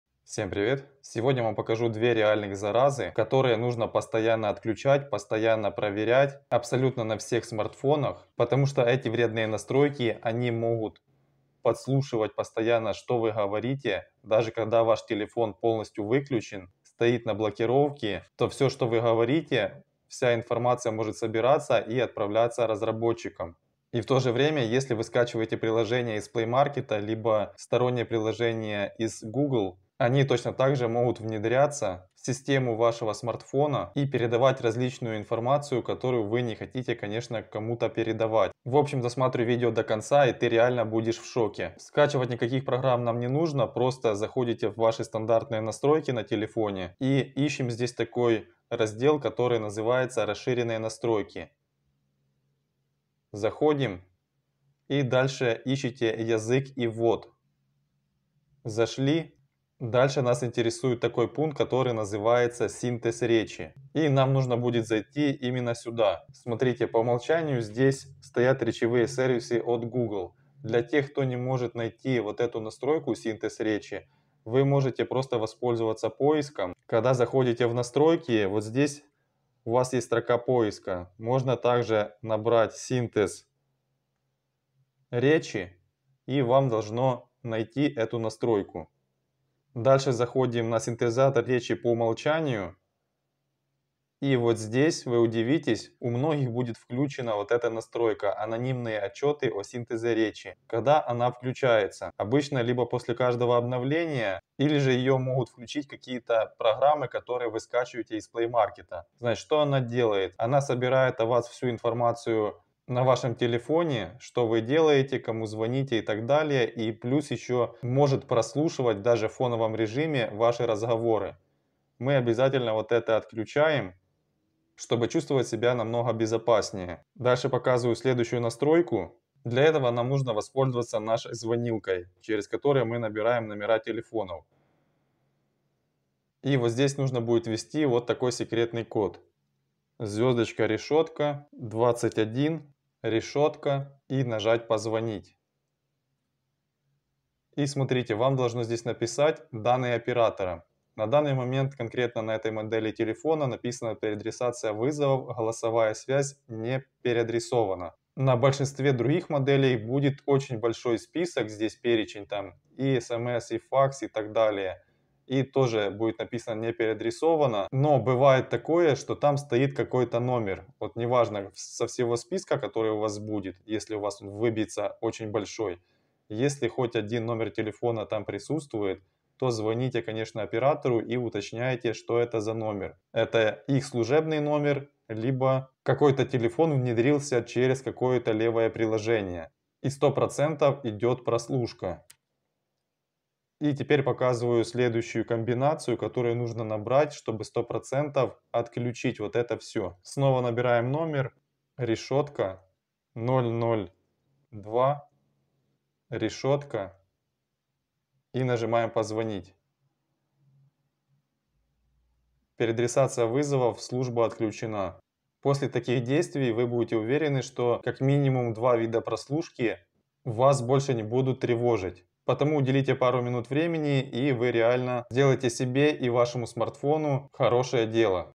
СРОЧНО ОТКЛЮЧИ 2 ВИДА СКРЫТОЙ ЗАРАЗЫ на ТЕЛЕФОНЕ Андроид❗ Как отключить прослушку ?
СРОЧНО ОТКЛЮЧИ 2 ВИДА СКРЫТОЙ ЗАРАЗЫ на ТЕЛЕФОНЕ ❗

⏩Сменил Регион MIUI На ИНДИЯ и ОФИГЕЛ! 😱